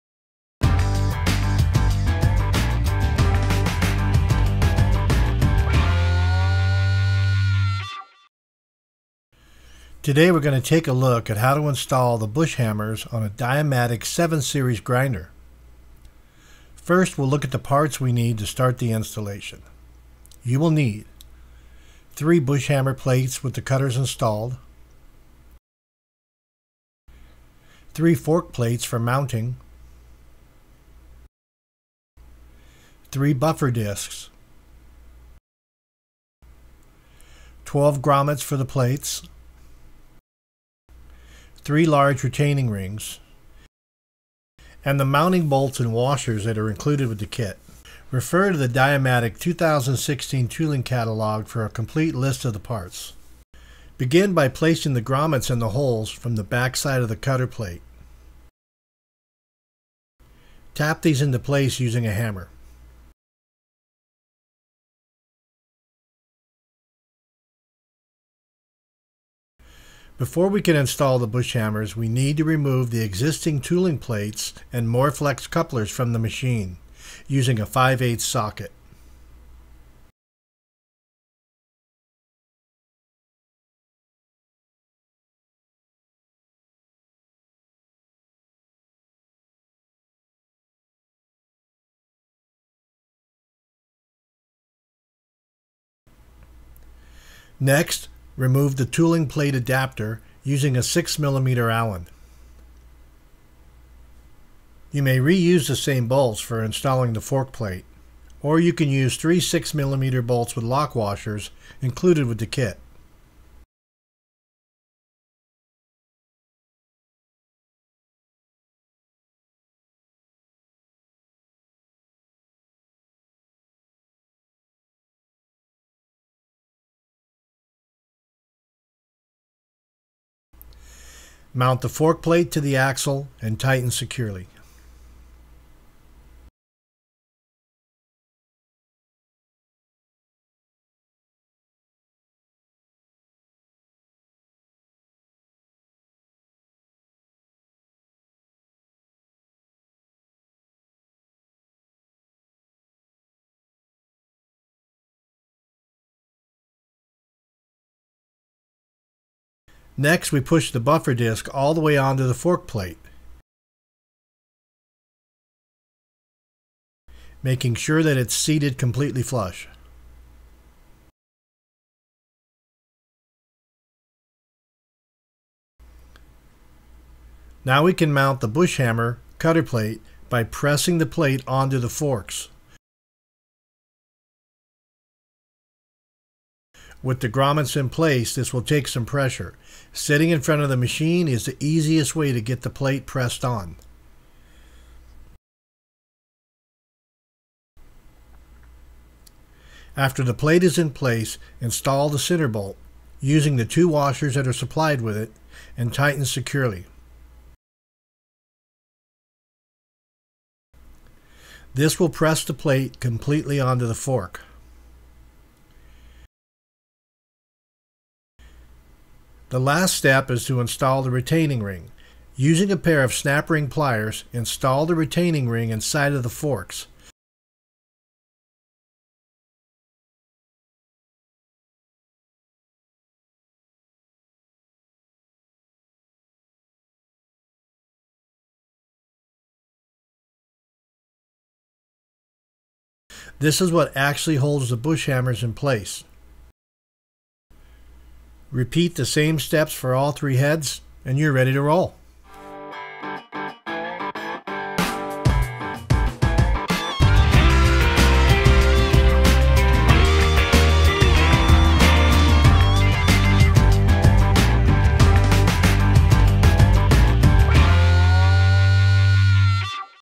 Bush Hammer Installation
How to install the bush hammers on a Diamatic 7 Series grinder